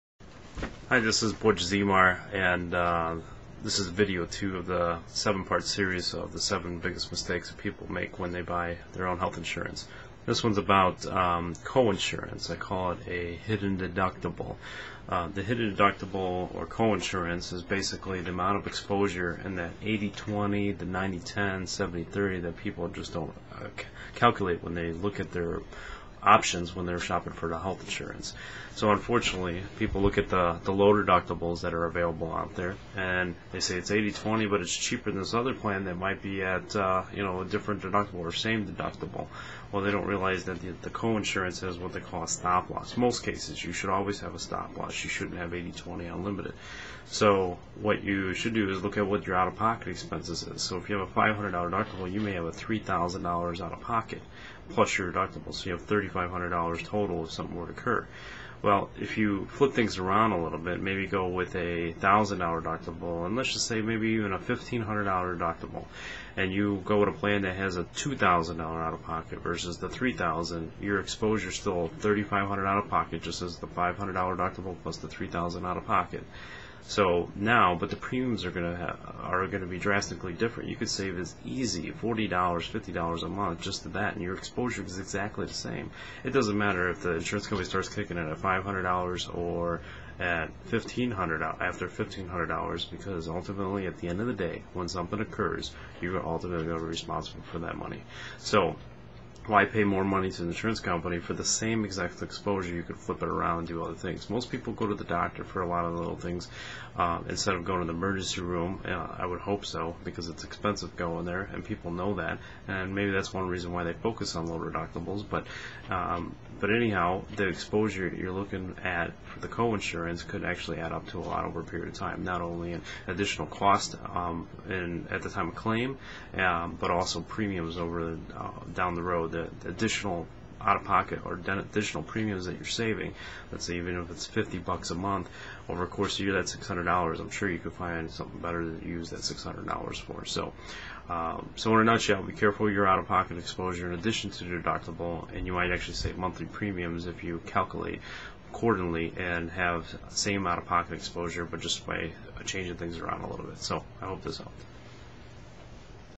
Seven Biggest Mistakes People Make With Health Insurance - Part Two Co-Insurance
Most people spend more than they need when it comes to Health Insurance premiums.  Your total out-of-pocket exposure is more important than you deductible or co-pay.  At the end of the day, at the time of claim, it's about how much you will be responsible for.  Claim time is no time for surprises.